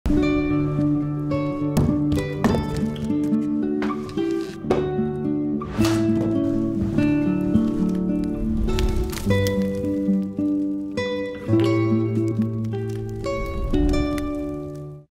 Joy to the Work // That cozy fire won't build itself…
The holiday season is a whole lot of hard work. Between stringing up lights to make spirits bright, building cozy fires to warm the whole family, and all the ways we work to create joy, Wolverine sees and celebrates the holiday hustle. Here's to those who work hard to make Christmas merry!

Shop gifts and gear for making holiday cheer at Wolverine.com